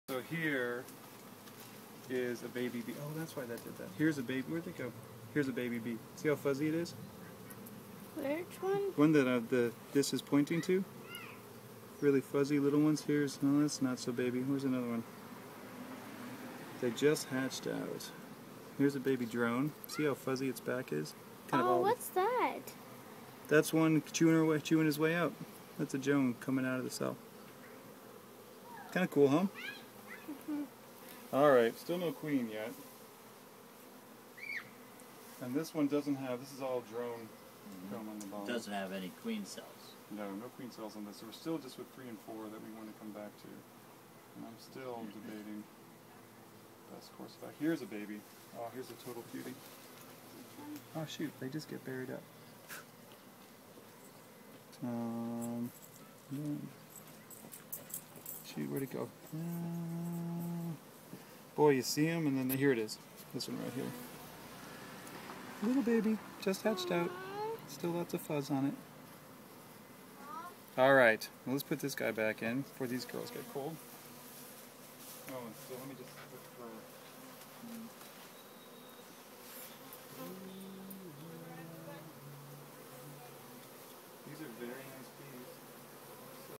Home beekeeping with Mace Vaughan of Xerces.org
Mace Vaughan, Pollinator Program Director of The Xerces Society helps a home beekeeper with his hive.